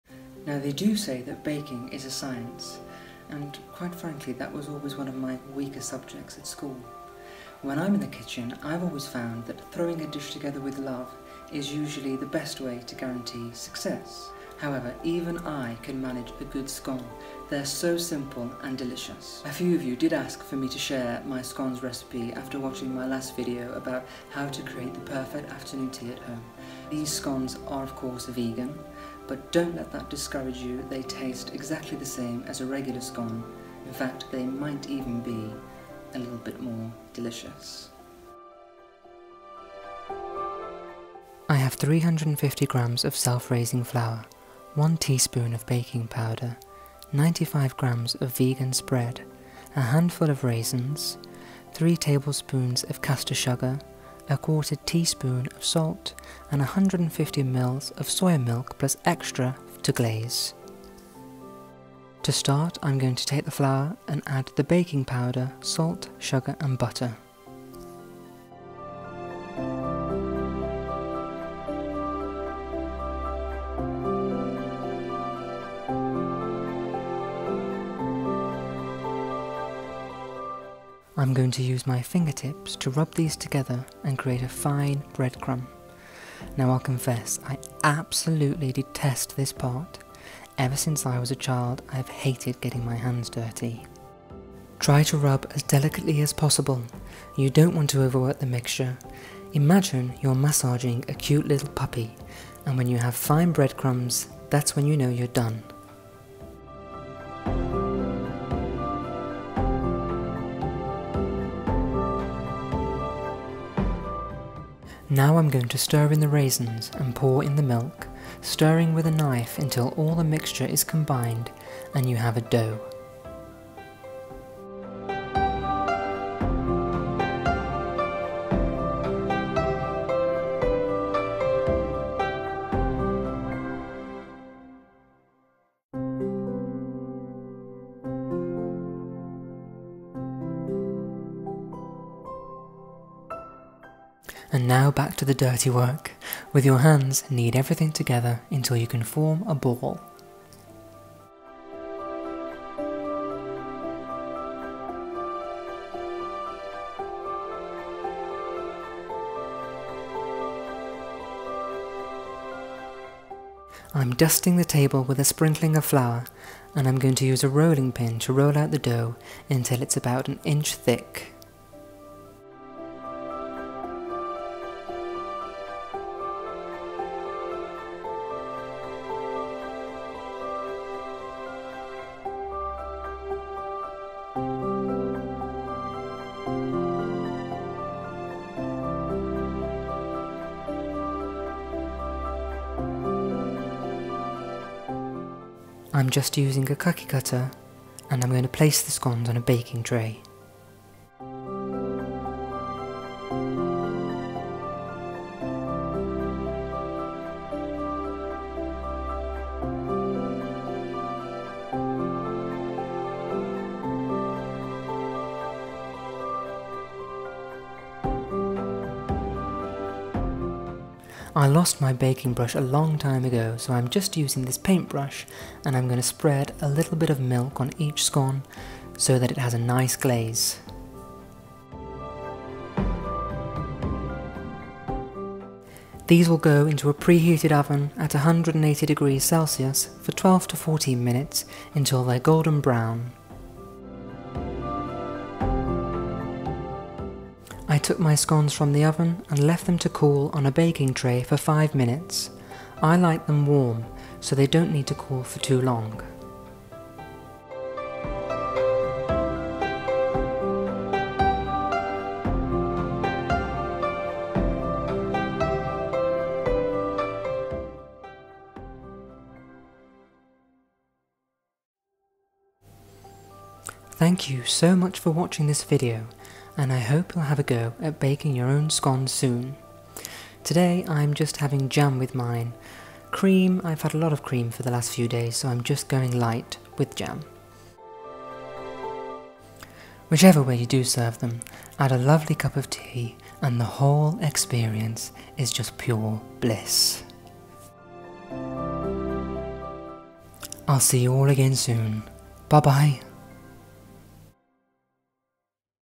Nicolas Fairford Scone Recipe
In this video I show you how to bake my very easy scones! They are vegan but taste just as good as regular scones.